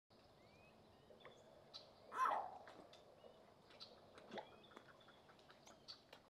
Hear the Green Heron Sound!
Green Herons are some of the more colorful herons, found mostly in wetlands. Their signature "Skeow" call is a trademark sound of the marshes. This was recorded at Everglades National Park on the Anhinga Trail, where you can find many bird species along a beautiful boardwalk path. Filmed on my Canon R6 camera, RF 600mm F11 lens, RODE VideoMic Pro+